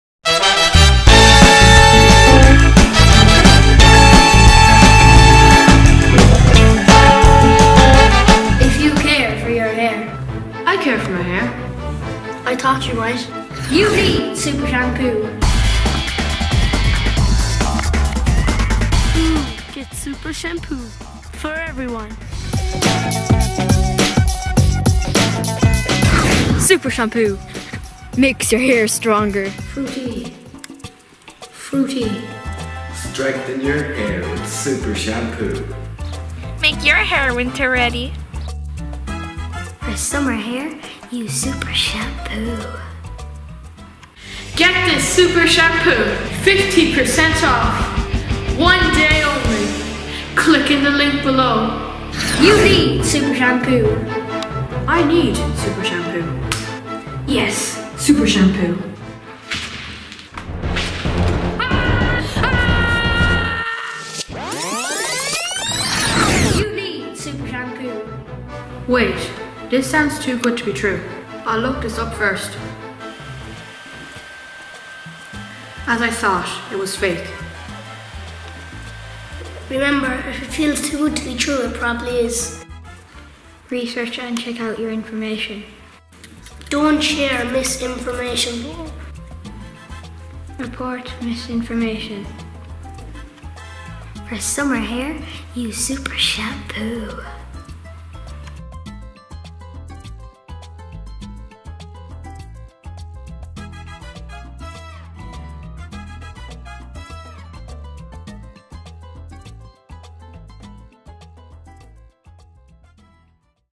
SUPER SHAMPOO
FAKE NEWS CAN DOMINATE THE INTERNET TODAY FROM ADVERTISING TO BREAKING NEWS . SEE WHAT HAPPENS TO ONE CHILD WHO IS CONVINCED BY AN ADVERT THAT HE NEEDS A PARTICULAR PRODUCT.
LUCKILY WE ALSO SEE WHAT MAY HAVE HELPED IN AVOIDING A HAIR DISASTER !!!